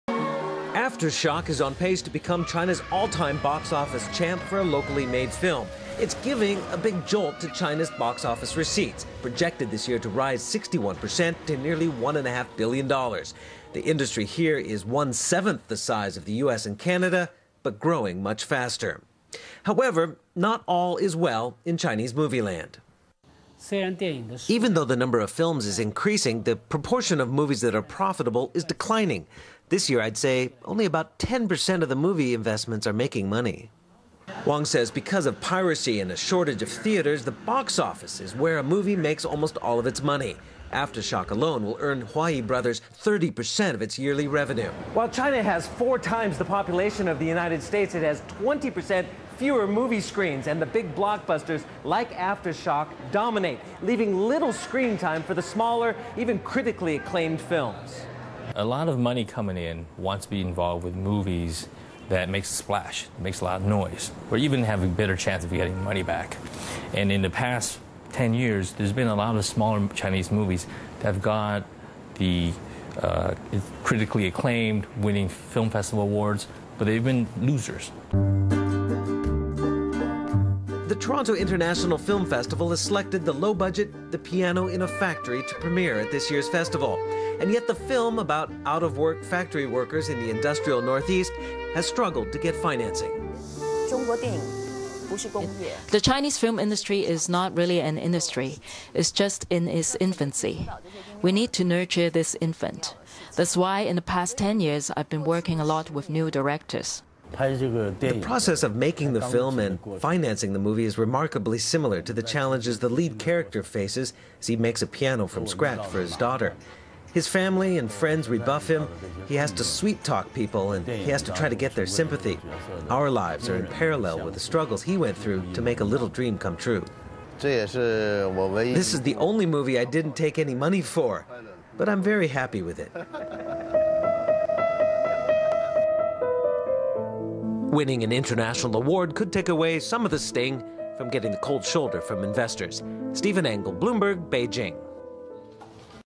China's Film Industry Faces Domestic, Foreign Challenges: Video
Aug. 9 (Bloomberg) -- Bloomberg's Stephen Engle reports on the challenges facing China's film industry.
     China is encouraging the development of its film industry to help local movie makers compete with overseas producers such as Time Warner Inc. China's box office receipts are projected to rise 61 percent to 10 billion yuan this year, the State Administration of Radio, Film and Television projected in May. (Source: Bloomberg)